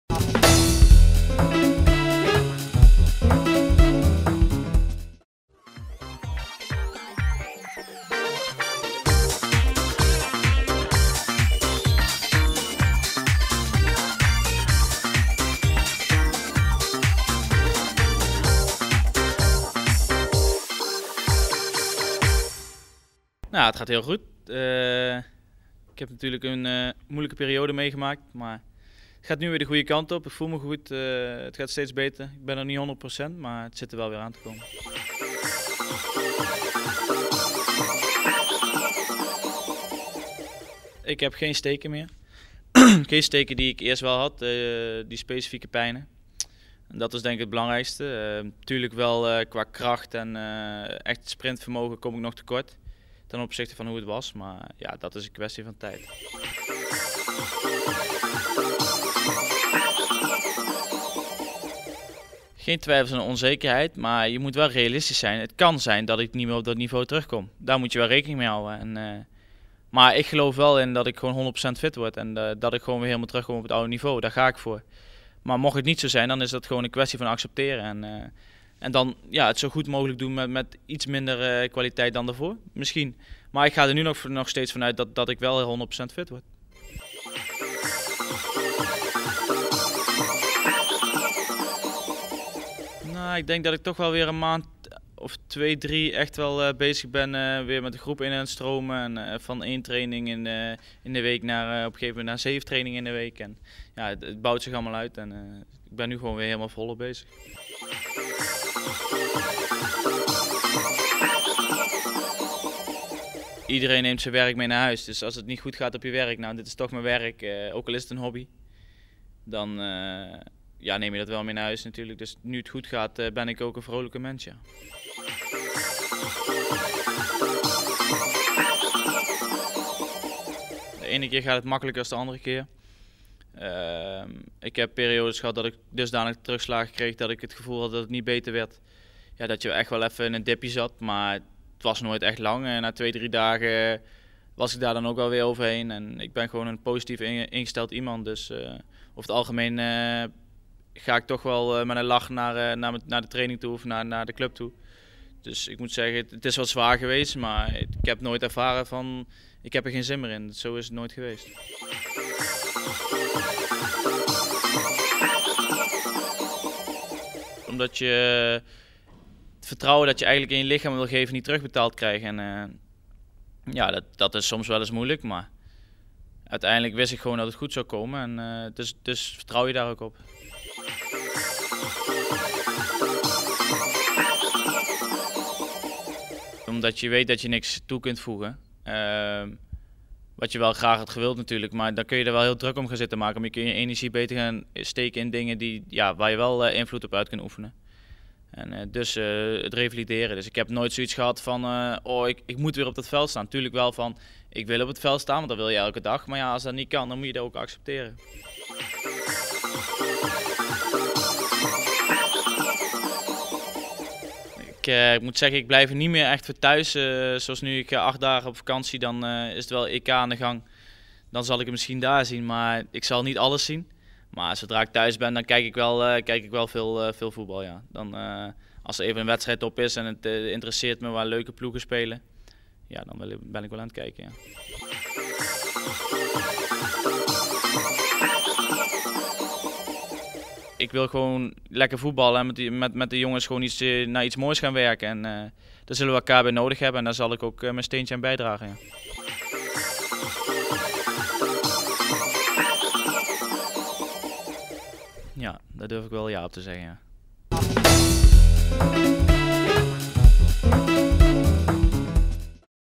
AZ'er Schaars: 'Misschien niet terug op oude niveau'
Dit seizoen speelde hij geen wedstrijd voor AZ, vanwege een chronische enkelblessure. Begin april maakte Stijn Schaars zijn rentree in Jong AZ. De 24-jarige middenvelder kwam op 15 april 2007 tegen zijn oude club Vitesse voor het laatst in actie. Bastian Ramler zocht hem voor Sportweek TV op in Alkmaar en ontmoette en positief gestemde Schaars. Ook in het blad deze week een uitgebreid interview met de zesvoudig international.